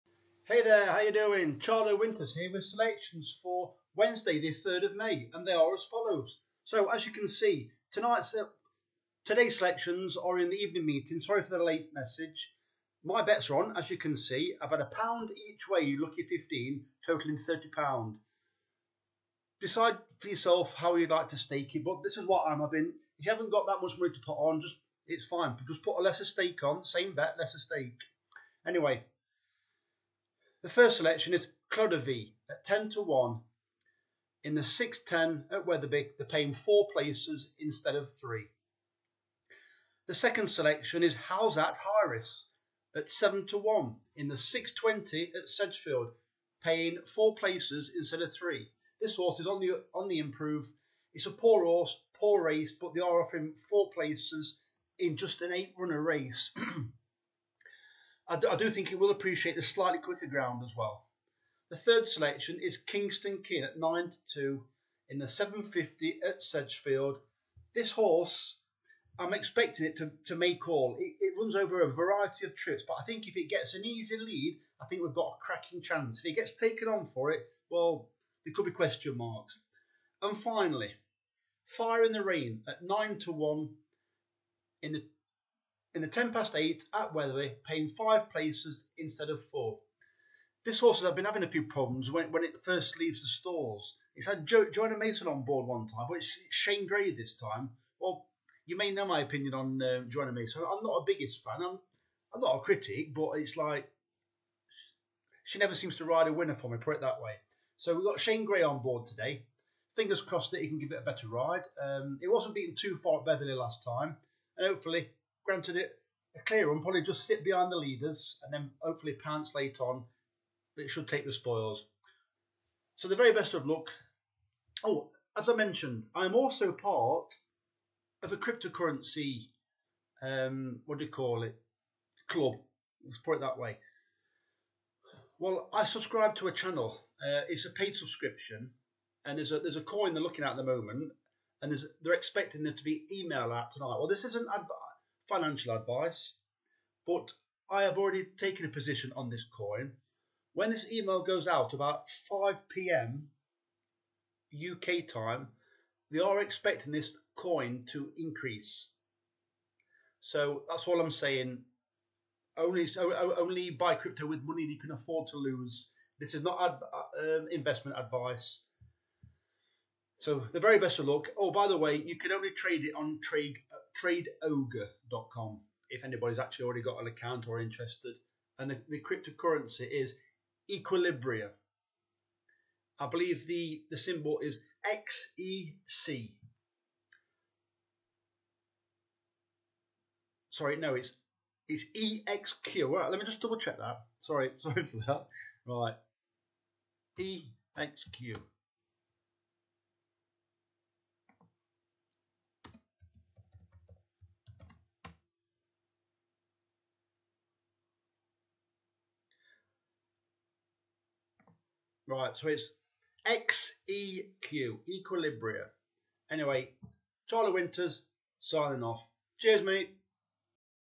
Today's £1 E.W Lucky 15 TIPS Are READY! (Skybet Extra Place Races)
3 out of the 4 races are skybet extra place races...  Good luck Charlie